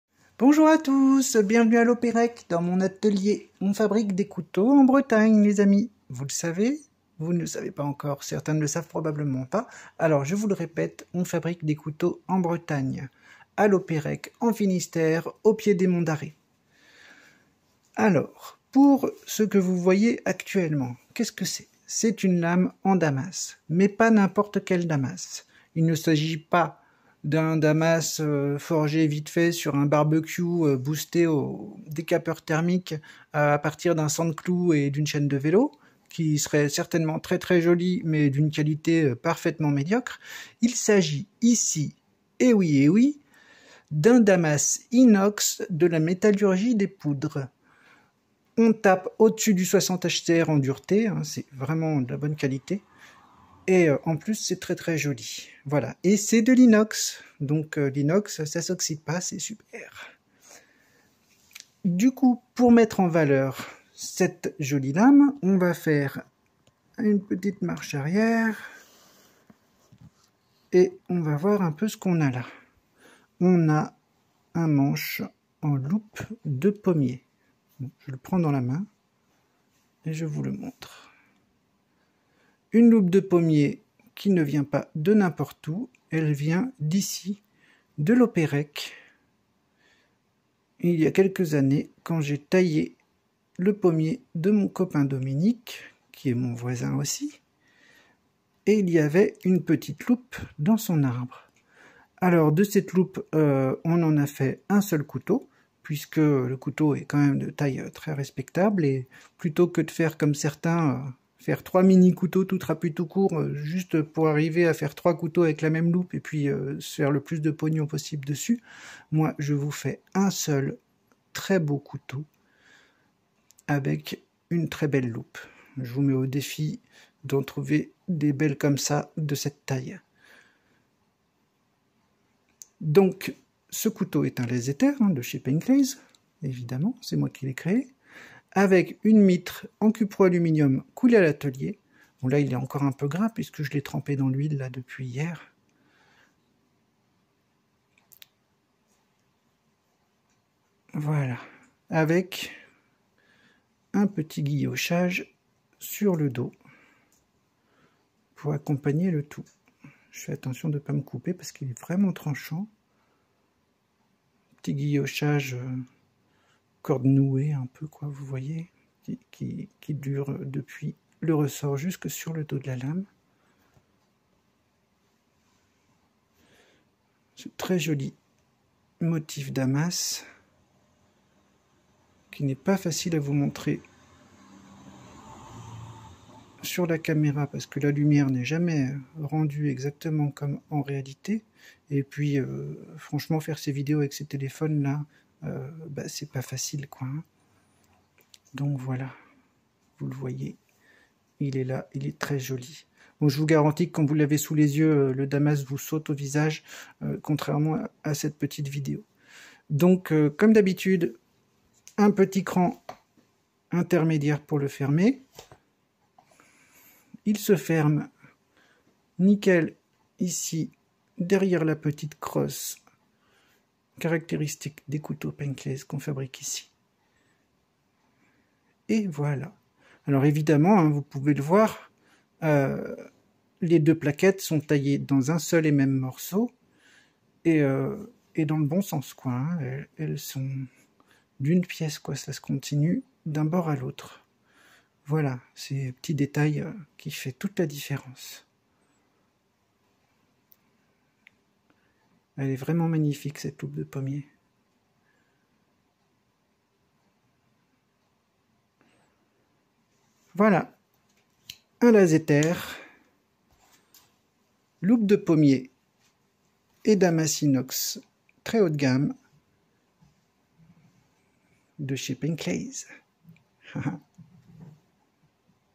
Damas inox, métallurgie des poudres et loupe de pommier sur mitre cupro-aluminium
Couteau breton le "Lazhetaer"
Lame de 11,5 cm en damas inoxydable issu de la métallurgie des poudres RWL34 et PCM27.
Manche en loupe de pommier de Lopérec stabilisé sous vide
Mitre soudée à l'argent en cupro-aluminium